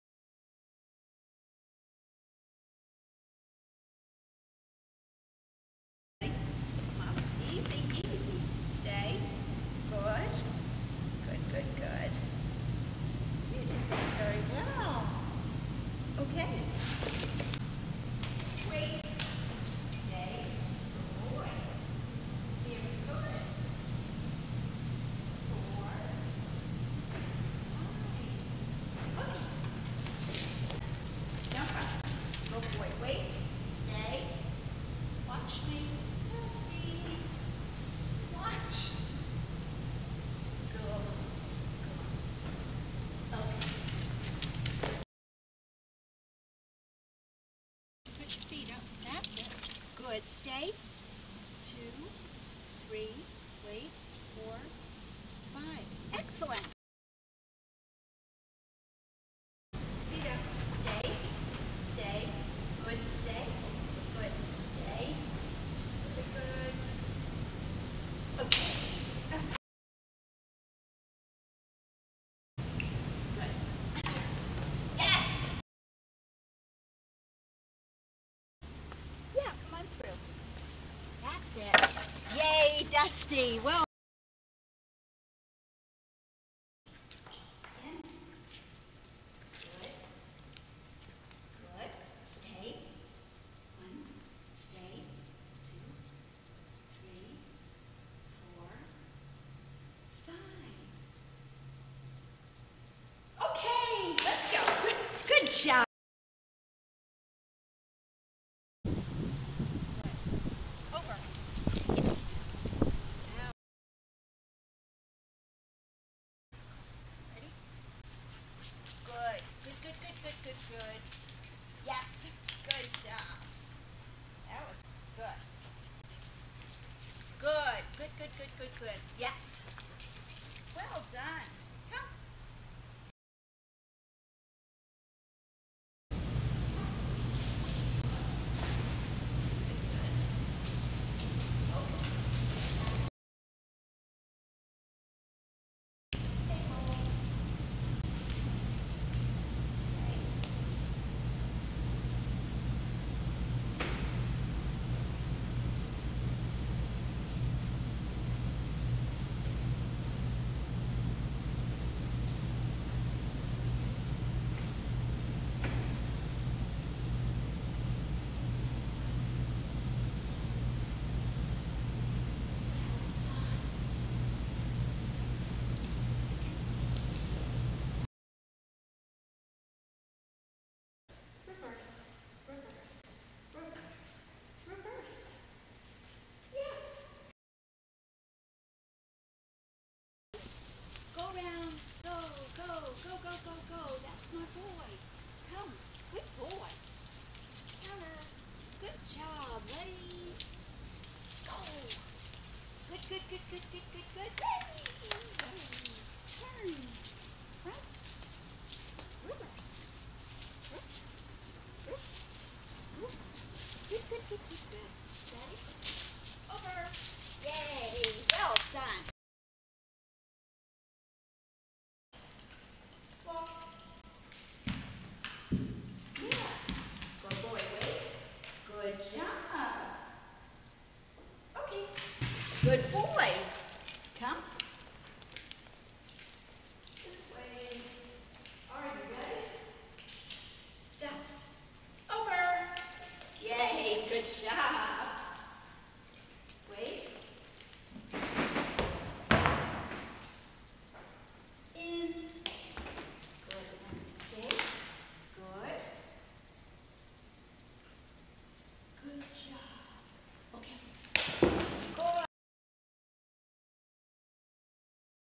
Dusty - Dog Parkour Performance (Novice Level)
This entry qualified in September of 2015 when dog parkour was a new thing and IDPKA was the only parkour venue. If I were to do this now, I would do MUCH BETTER spotting! In 2016 I started All Dogs Parkour, and safe handling practices are paramount. The sport has grown a lot in the past several years and continues to increase in popularity.